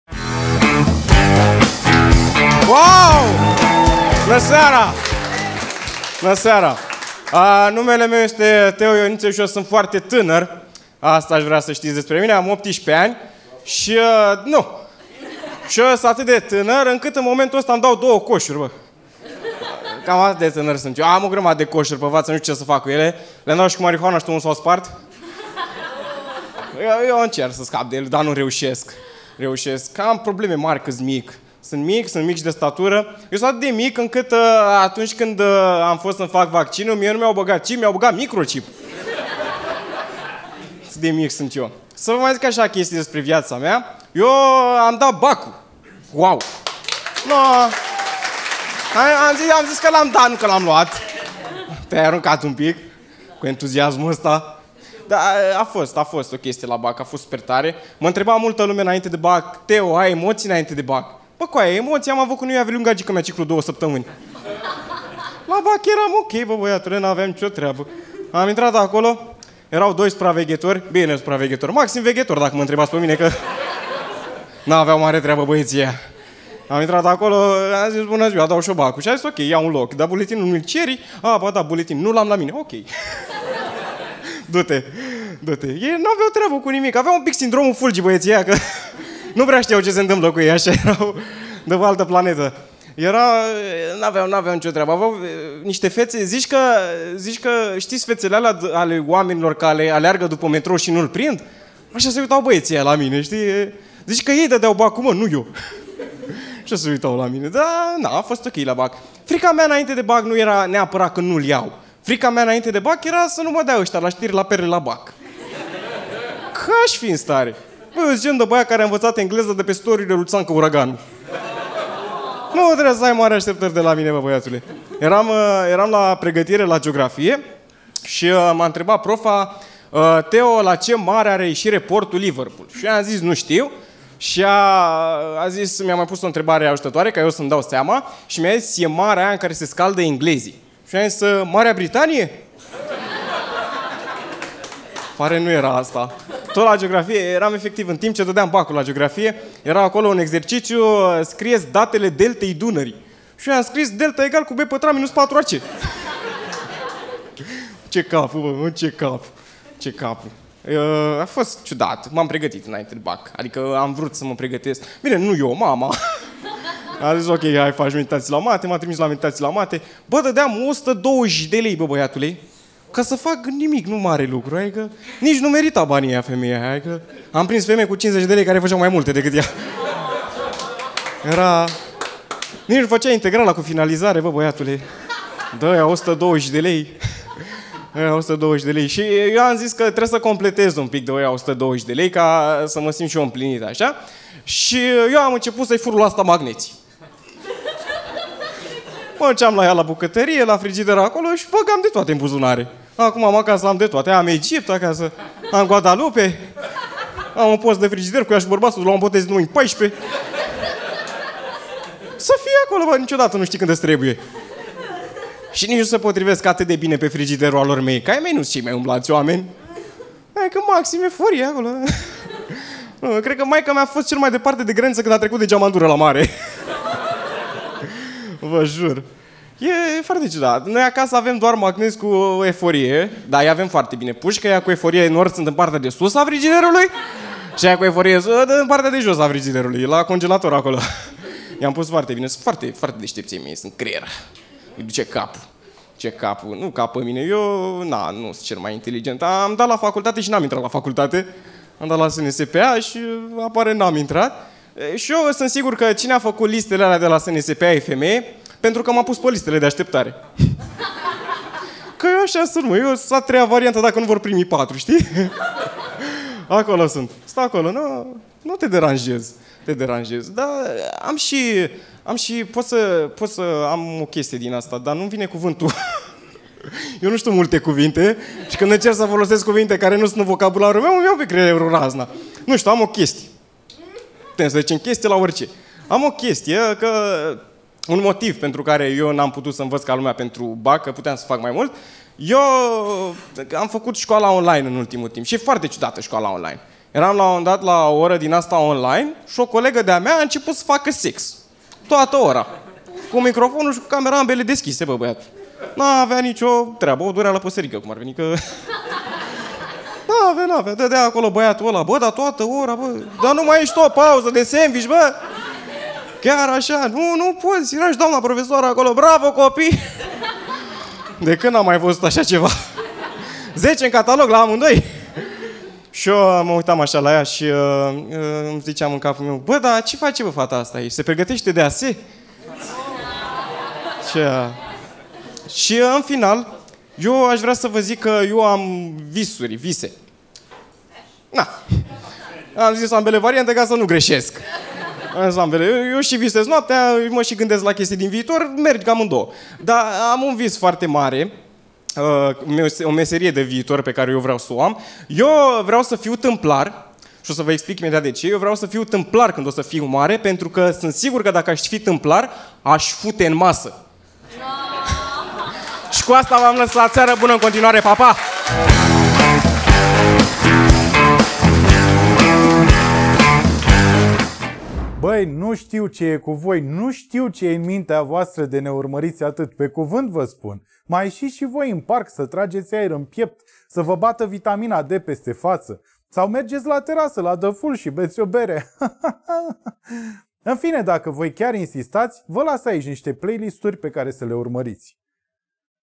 5 MINUTE cu Teo Ioniță | ”Am luat BAC-ul!” (Stand-up Comedy)
Începem seria ”5 MINUTE” de stand-up cu Teo Ioniță. Teo e mic, e foarte mic, ne zice și el asta în bucata lui de stand-up, tot în acest video se laudă și c-a dat BAC-ul, nu știm dacă l-a și luat, dar până la urmă, cine suntem noi să judecăm.
E totodată și prima bucată de stand-up pusă de Teo Ioniță pe net, noi vrem să-l susținem și să-i urăm multă baftă la facultate, să iei bursă mamă, să înveți bine și nu uita Teo, ai, n-ai licența... bucățile pe net același număr de vizionări au!

Pe Teo Ioniță îl găsiți ori pe Calea Victoriei 118, ori pe instagram:

Urmărește-ne pe Facebook și Instagram ca să știi exact ce și cum mai facem, pe și